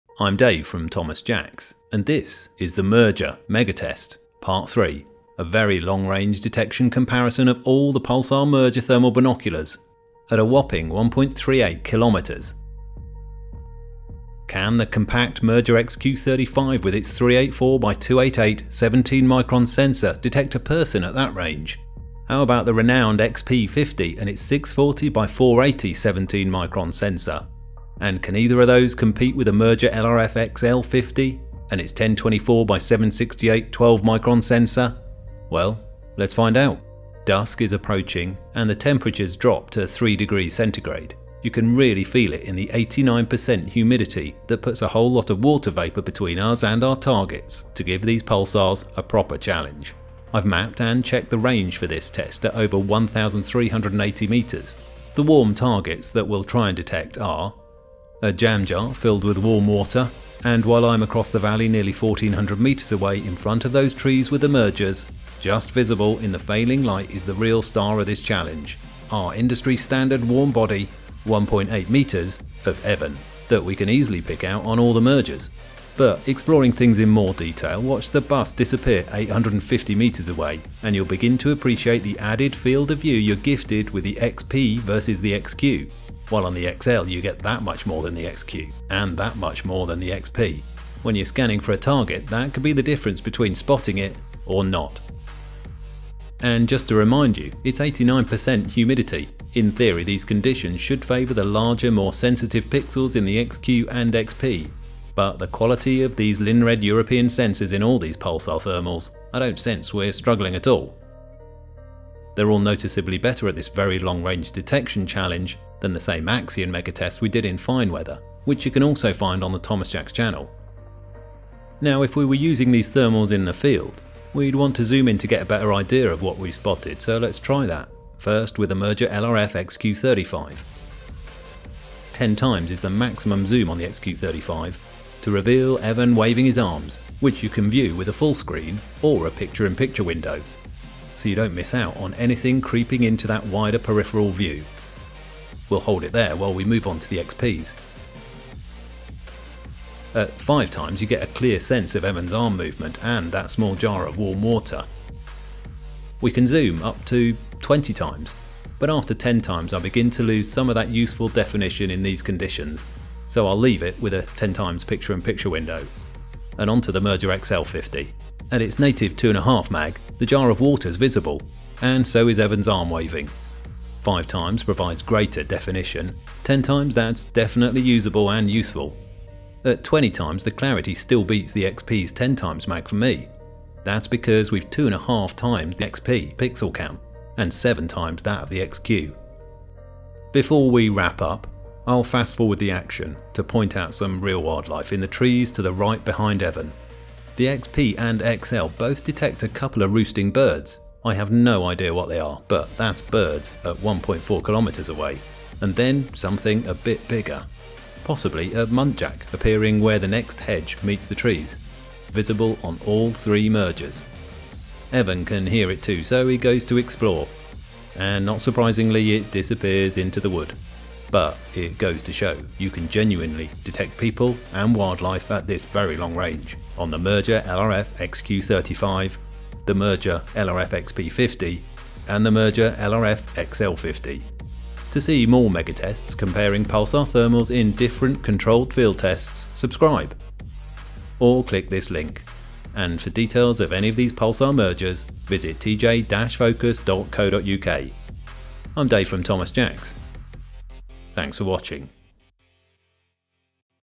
Pulsar Merger XQ35 vs XP50 vs XL50 at 1.38km. Detecting at a very long range.
Probably the BIGGEST benefit to owning any thermal device is detecting animals (or people) at a VERY LONG RANGE. In any weather, day or night, without you being spotted or disturbing the the wildlife you're hoping to see. And there is no better way of detecting at a very long range than the Pulsar Merger thermal binoculars. In this field test we compare the entry level Merger LRF XQ35, with the mid price Merger LRF XP50 and the premium Merger LRF XL50 at a whopping 1,380m... in 89% humidity.

See lots of side-by-side field test footage, showing you exactly how the different Pulsar Mergers compare. And there's a surprise at the end... it's not just a person we're able to spot at that BIG RANGE!

NOTE: To get a truer view of the image you'll see if you're serious about buying some Pulsar Merger thermal binoculars MAKE SURE YOU CHANGE YOUR YOUTUBE VIDEO SETTINGS (the cog below the video) to the best quality setting available. And to see the very small objects 1.4km away, that are obvious through the eyepiece, ideally watch this on a PC rather than your phone.

For more information on the Pulsar Merger LRF XQ35, Merger LRF XP50 or Merger LRF XL50 visit tj-focus.co.uk